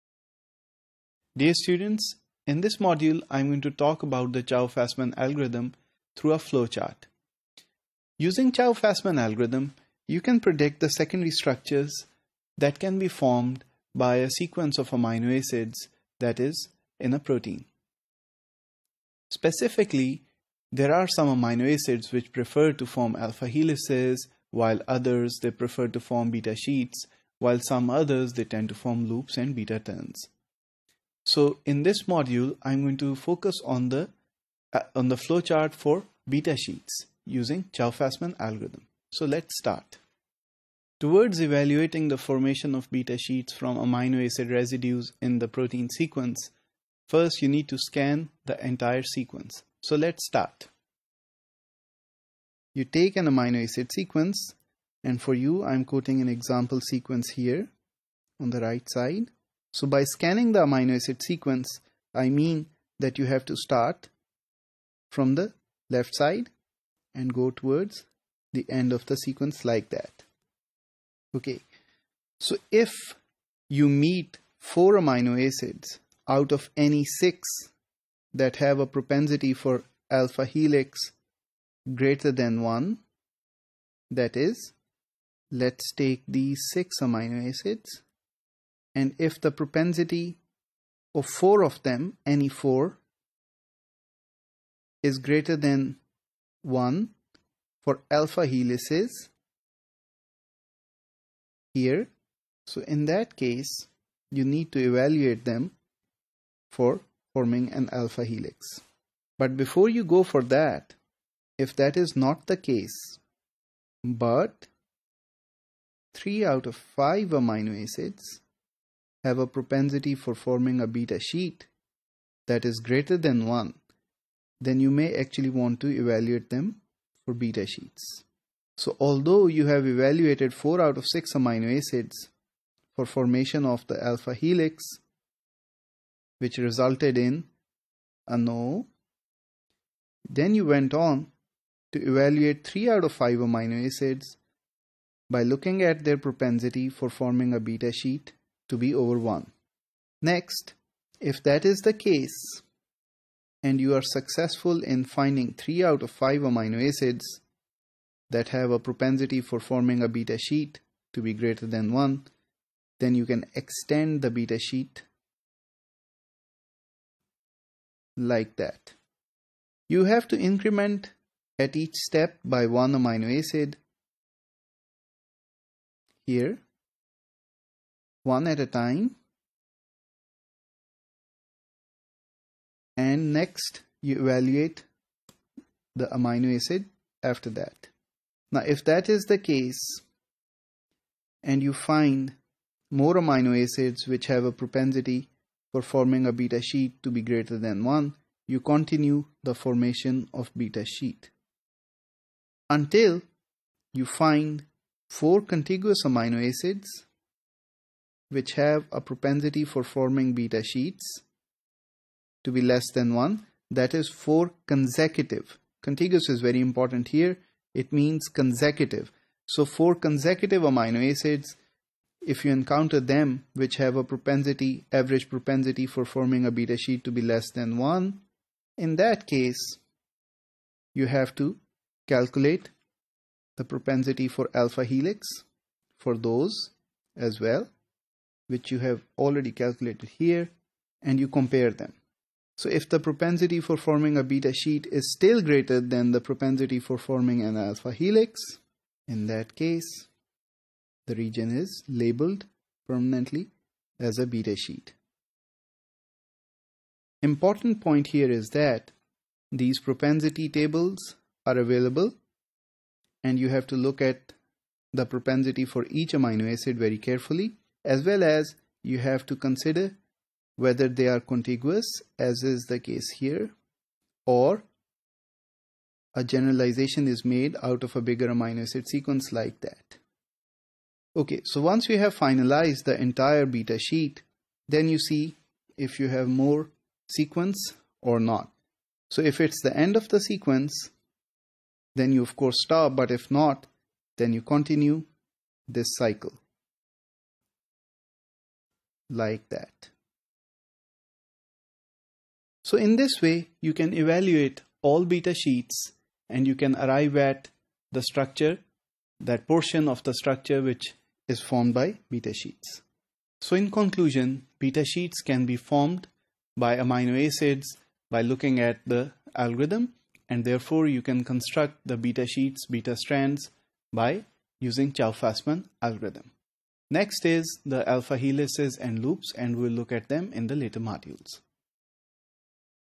Topic 212 (BIF401 - Bioinformatics I)
Course Code: BIF401
Course Name: Bioinformatics I
Instructor: Dr. Safee Ullah
Topic: Chou Fasman Algorithm - Flowchart I